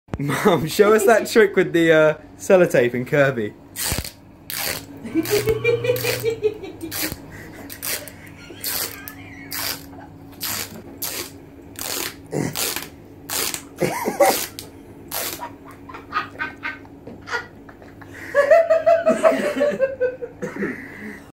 Sellotape Confuses Cat
Our cat, Kirby, has a weird reaction to the sound of sellotape.